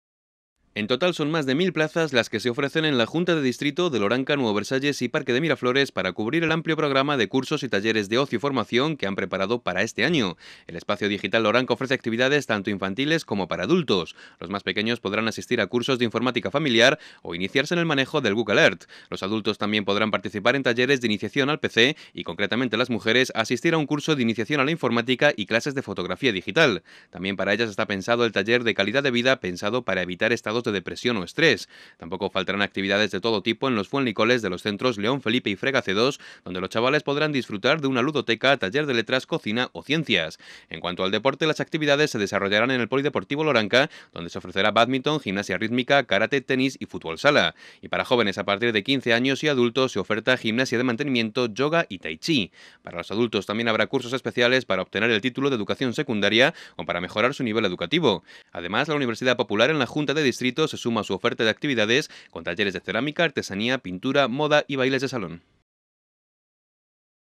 Cursos, talleres y actividades deportivas en la Junta de Distrito de Loranca en Fuenlabrada
Entre el 20 y 24 de septiembre todo el que quiera podrá apuntarse a la amplia oferta de cursos y talleres y ofrece la Junta de Distrito de Loranca, en Fuenlabrada. Iniciación a la informática, cursos de fotografía digital, radio y televisión y actividades deportivas son algunas de las posibilidades pensadas para todas las edades.